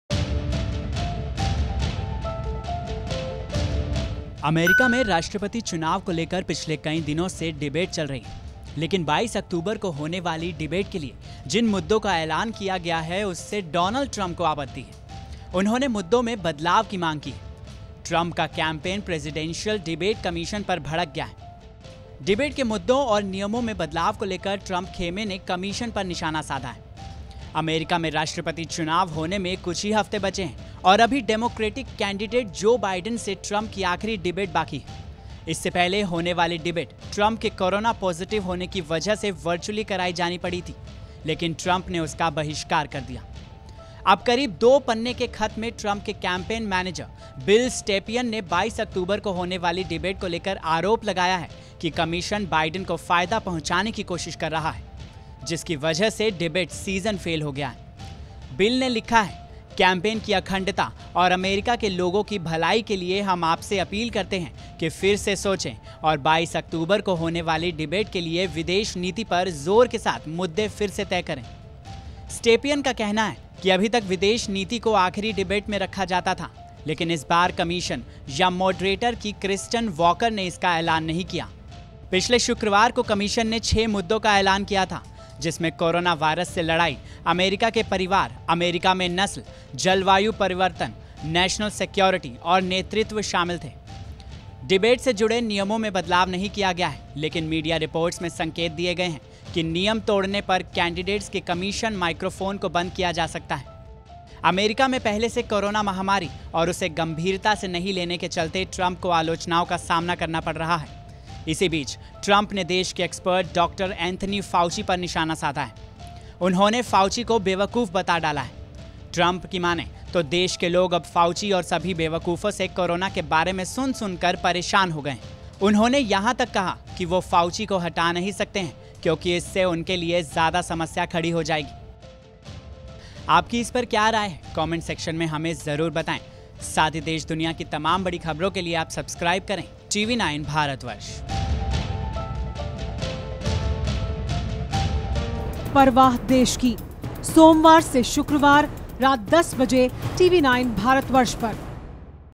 US Presidential Debate: आखिरी डिबेट के मुद्दों पर डोनाल्ड ट्रंप को आपत्ति, बदले जाने की मांग की
अमेरिका में राष्ट्रपति चुनाव को लेकर पिछले कई दिनों से डिबेट चल रही है. लेकिन 22 अक्टूबर को होने वाली डिबेट के लिए जिन मुद्दों का ऐलान किया गया है, उससे डोनाल्ड ट्रंप को आपत्ति है. उन्होंने मुद्दों में बदलाव की मांग की है. ट्रंप का कैंपेन प्रेसिडेंशल डिबेट कमिशन पर भड़क गया है. डिबेट के मुद्दों और नियमों में बदलाव को लेकर ट्रंप खेमे ने कमिशन पर निशाना साधा है.